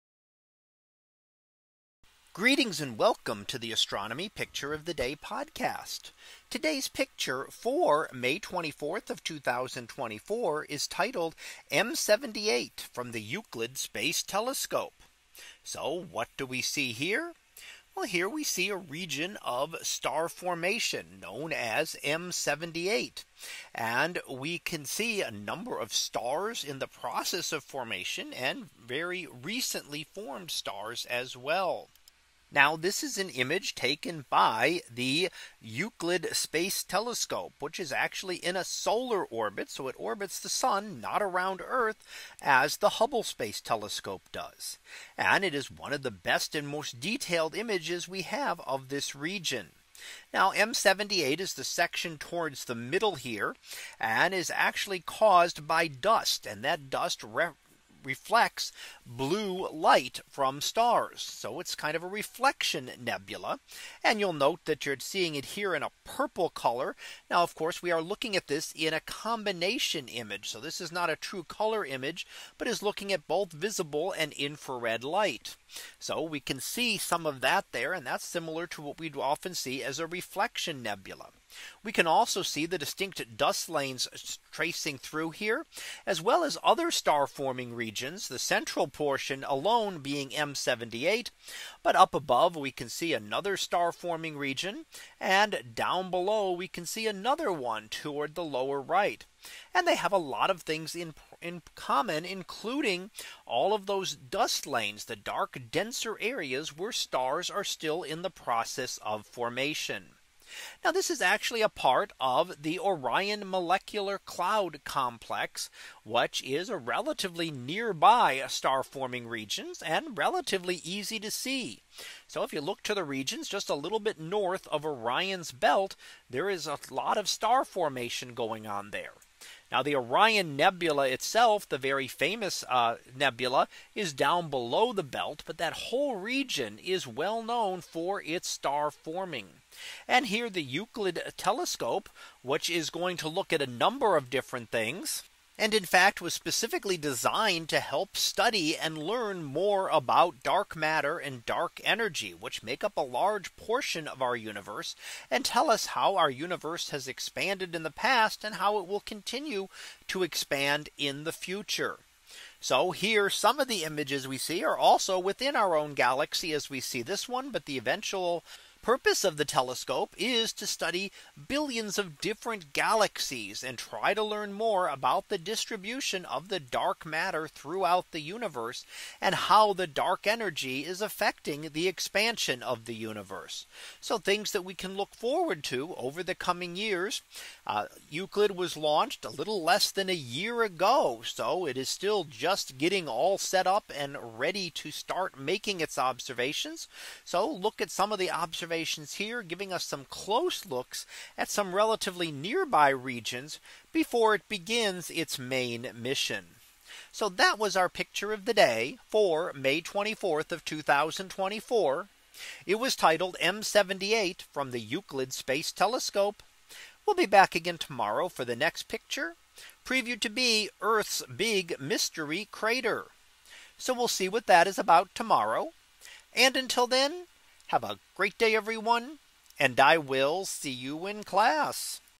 2024 May 24 - M78 from the Euclid Space Telescope
In today's image, we see the star forming region known as M78 as imaged in visible and infrared light from the Euclid Space Telescope. This telescope was launched last year and will be used to study the effects of dark matter and dark energy on our universe.